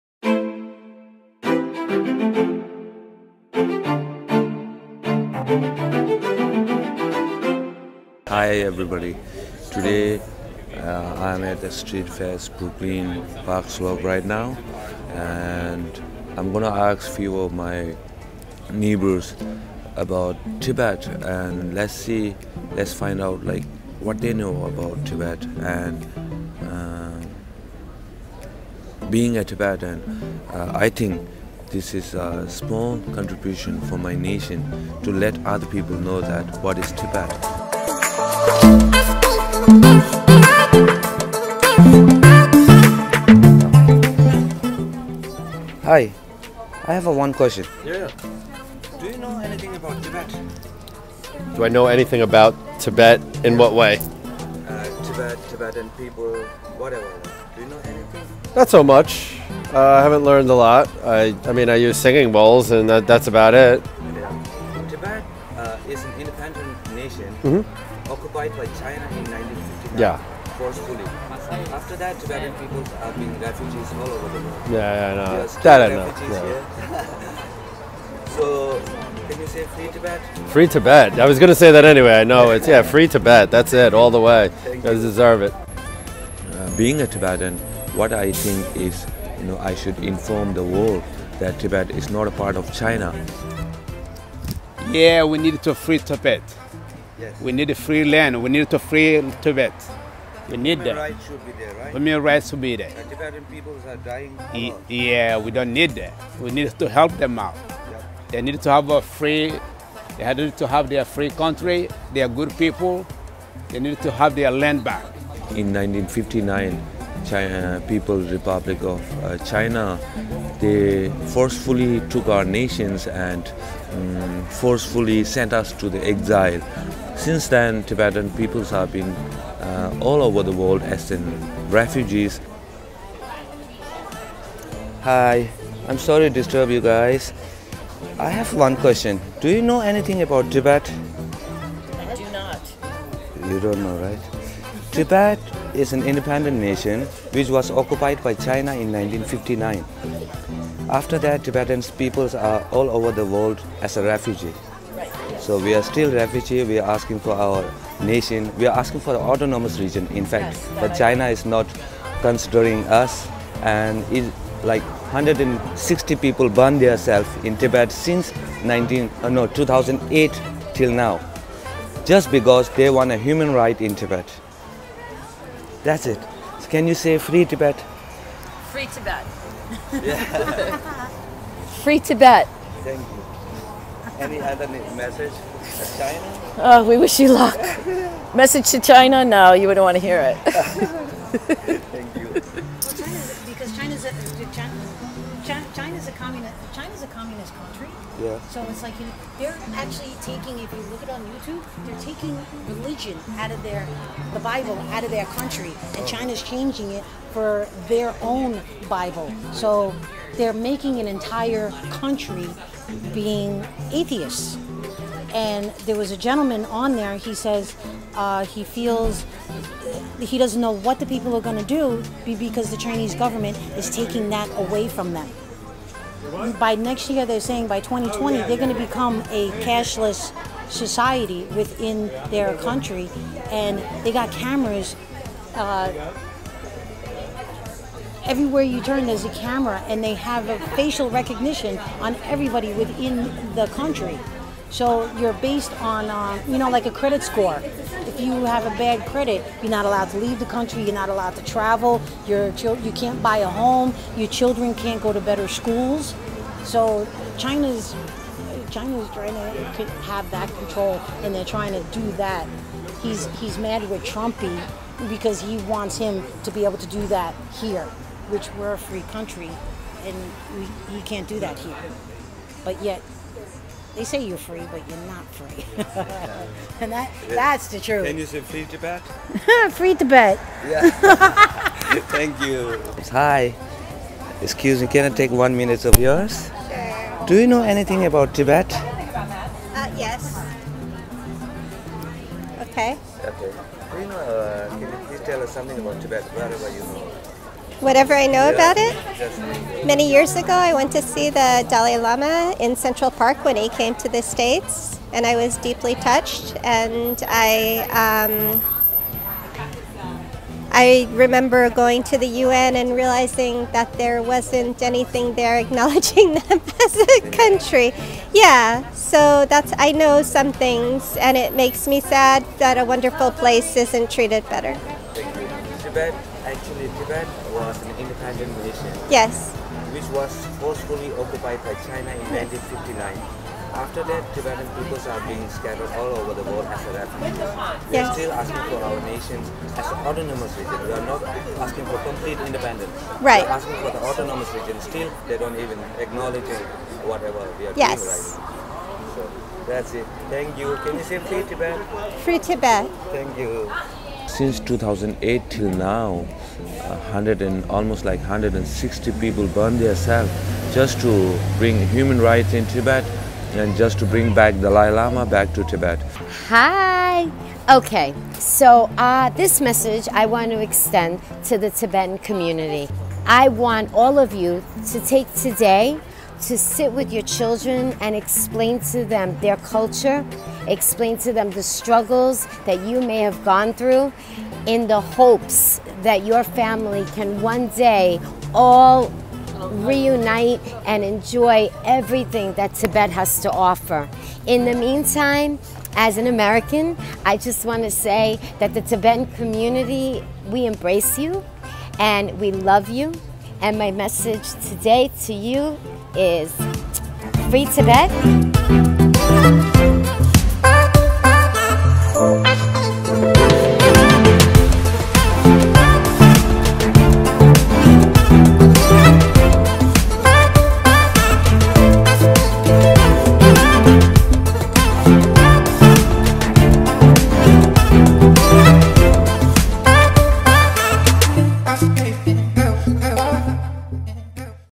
Asking New Yorker about Tibet ? // Tibetan vlogger //
TIBET IS NOT A PART OF CHINA

Camera i used for this v#log : Iphone8+
Background soung :